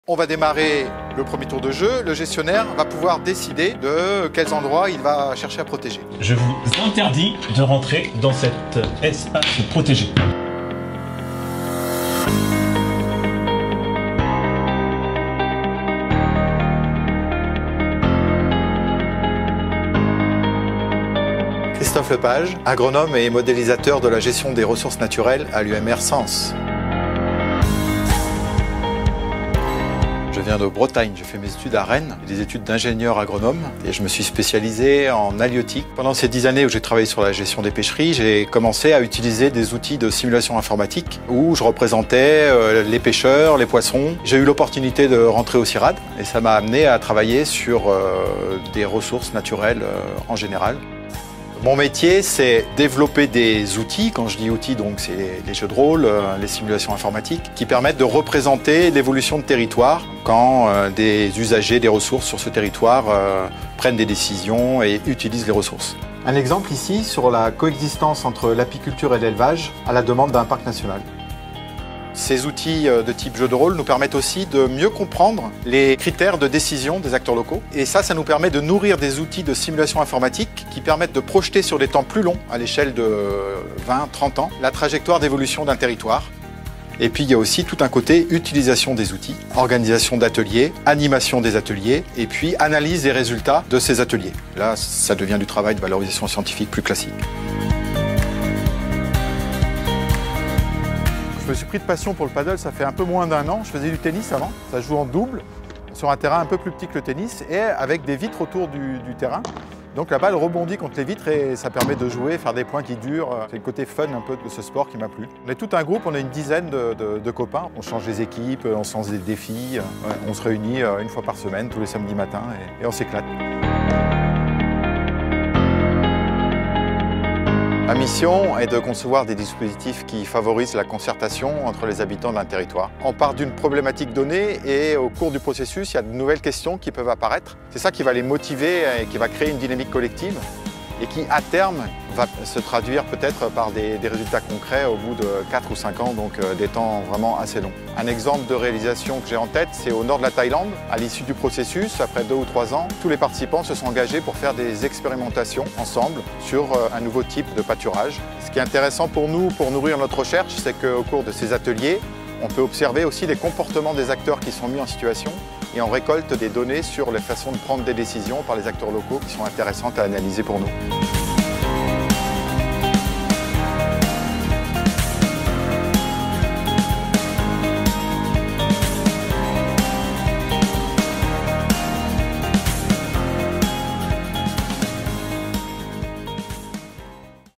Christophe Le Page, agronome et modélisateur de la gestion des ressources naturelles
Christophe Le Page nous emmène dans un voyage au cœur de son métier, de ses missions, de son quotidien et de son engagement... Cet agronome et modélisateur de la gestion des ressources naturelles développe des outils d'aide à la décision, à l'unité de recherche SENS, au Cirad.

Cette vidéo s'inscrit dans la série "Un talent, un métier" qui met en lumière les métiers originaux du Cirad.

N'hésitez pas à liker, partager et commenter cette vidéo !